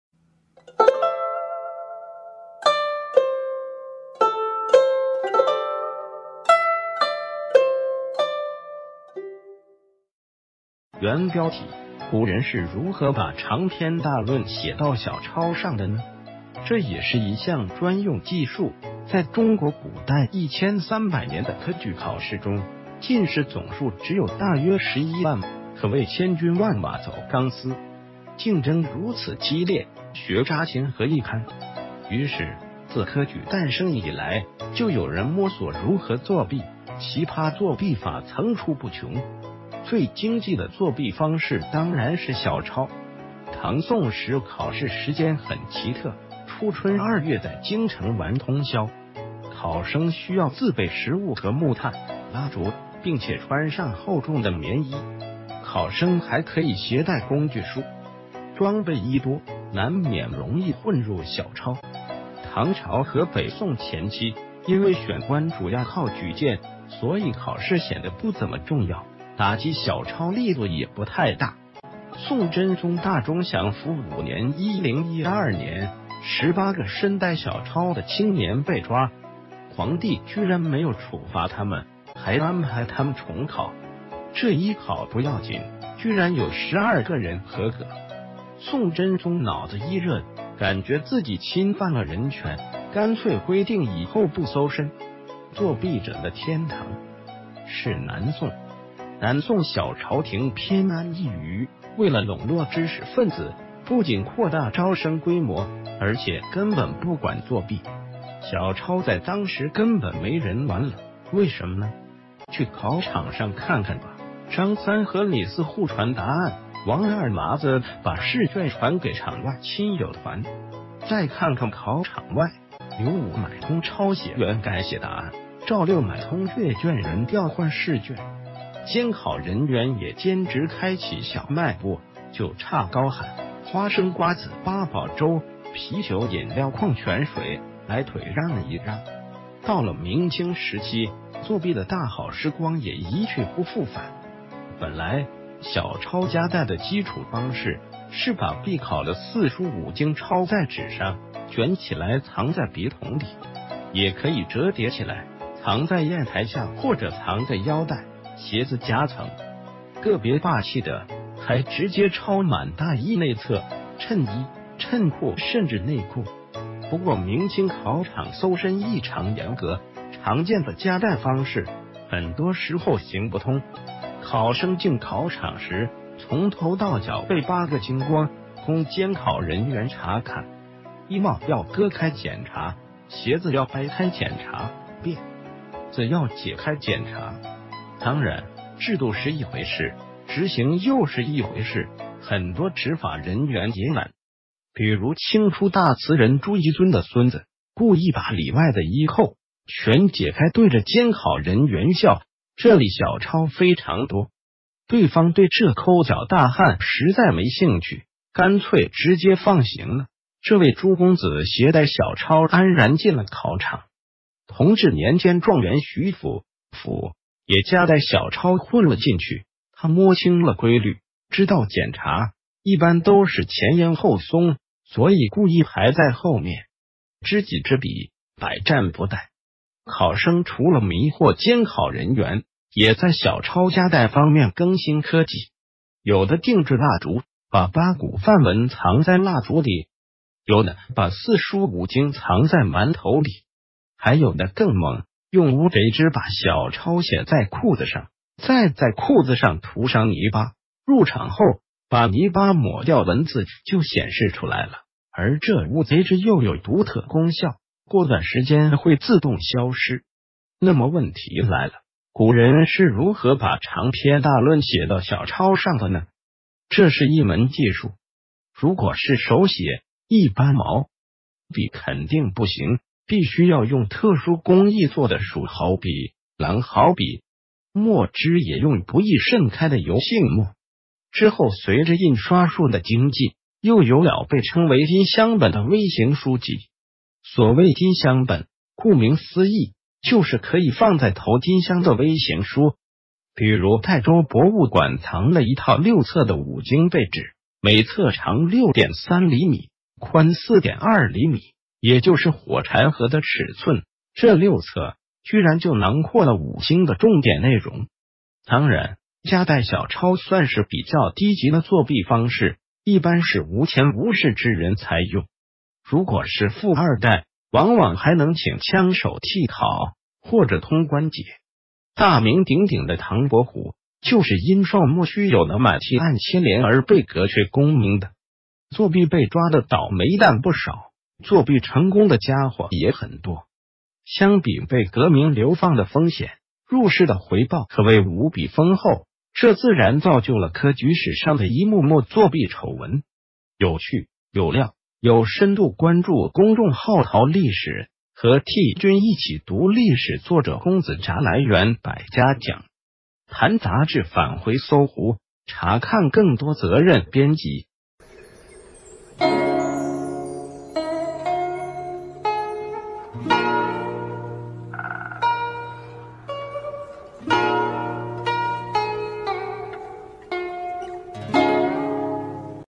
古人是如何把長篇大論寫到小抄上的呢？這也是一項專用技術_監考
原標題：古人是如何把...